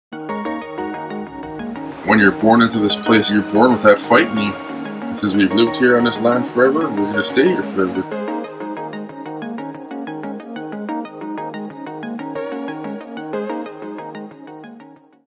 THIS PLACE...
Aeroclub Empordà takes you on a flight tour around la Costa Brava to discover the more preciate places...